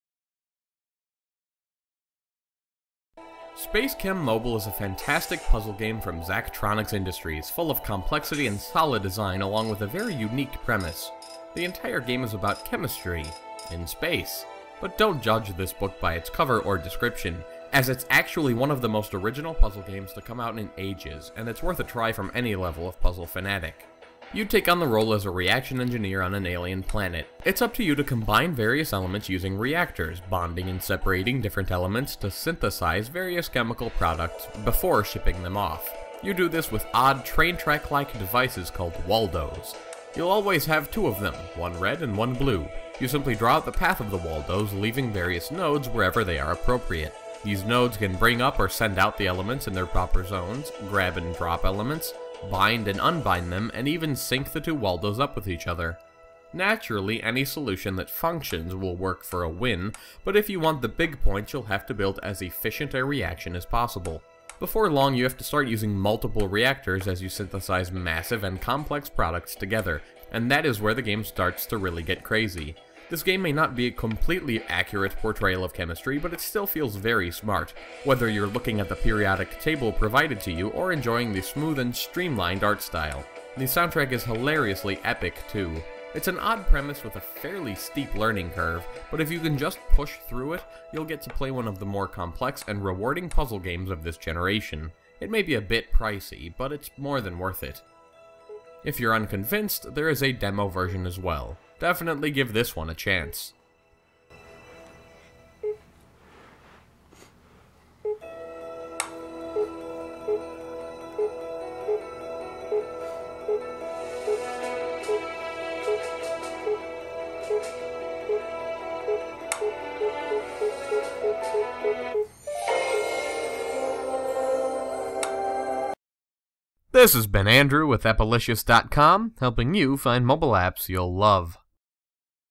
SpaceChem Mobile Android App Review - AndroidApps.com
Today: Chemistry has never been so much fun!

App: SpaceChem Mobile
Price: $5.99
The price depicted is at the time of this review, and could easily be changed.